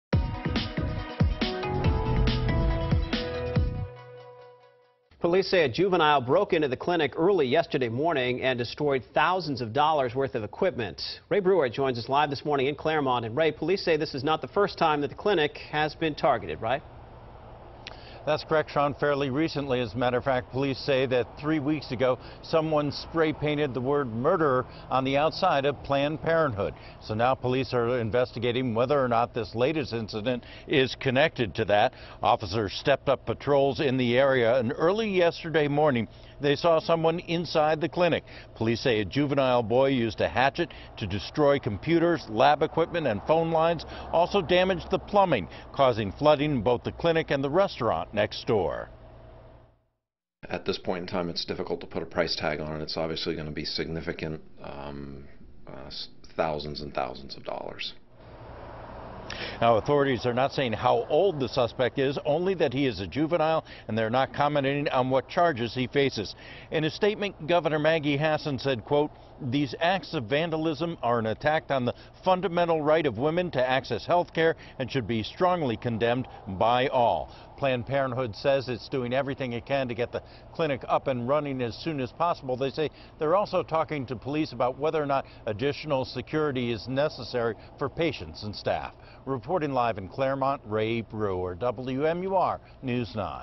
Planned Parenthood vandalized; juvenile in custody
he Planned Parenthood in Claremont is closed this morning after police say a juvenile broke in early Wednesday morning and used a hatchet to cause thousands of dollars in damage.